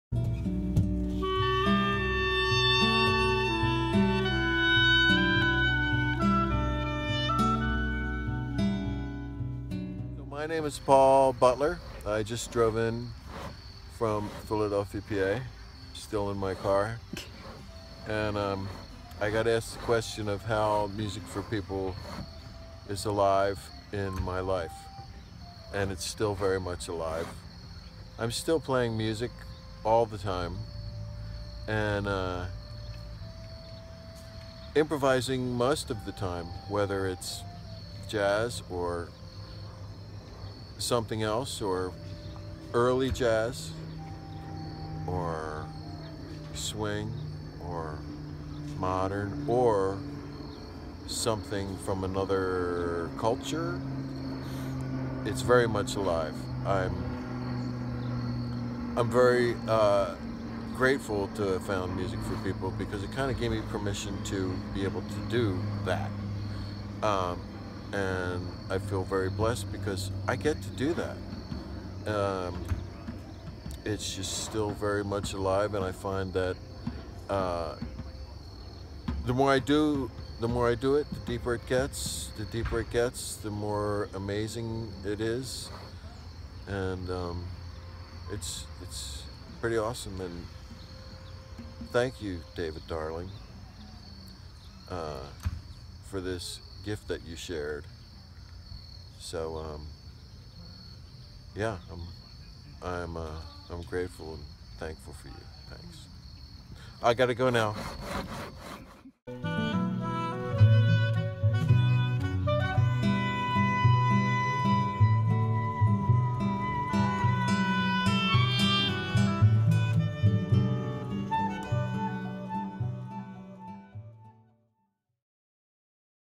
Thank You for This Gift - Paul Butler - Music For People Workshops
Paul expresses gratitude for the gift David Darling shared which helped him pursue a life in music.

For more information about Music for People and its music improvisation workshops